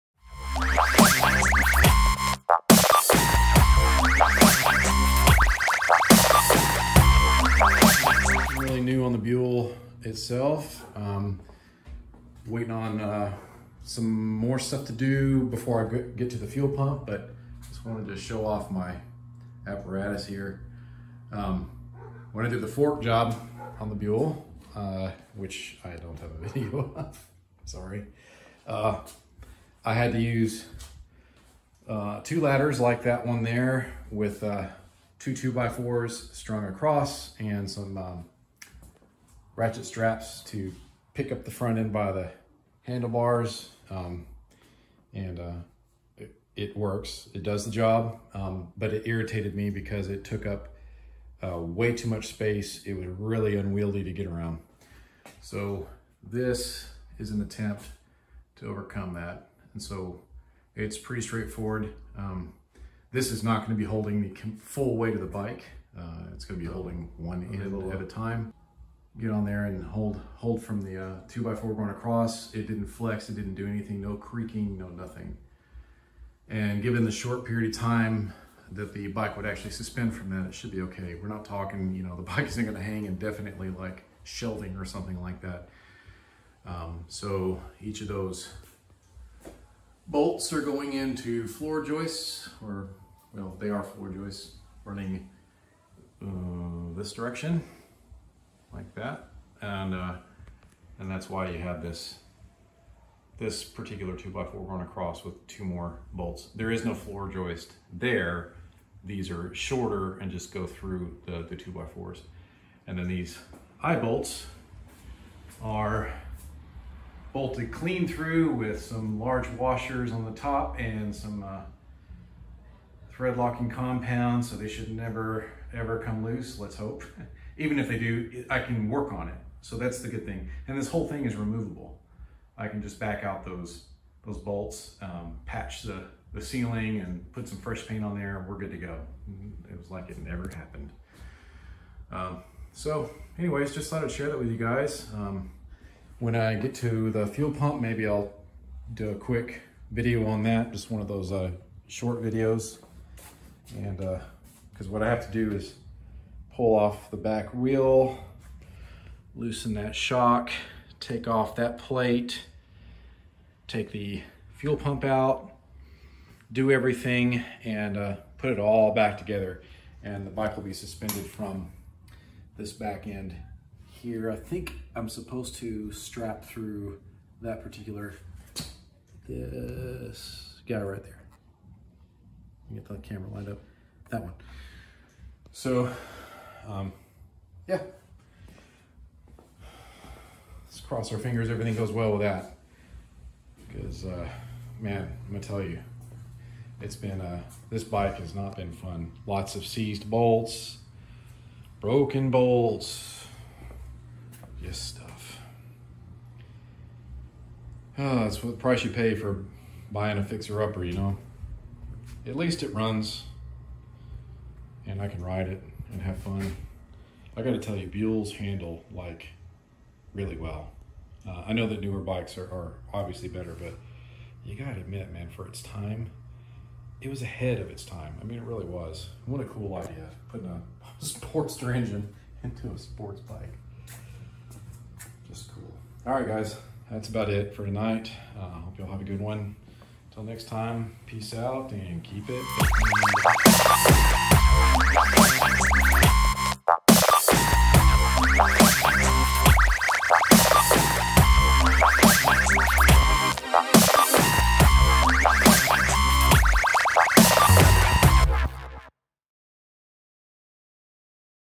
Build A Bike Hoist For Less Than $30
I needed a simple way to hoist my bike without breaking the bank. This was fast, cheap, and easy.

Hope this helps someone else out there.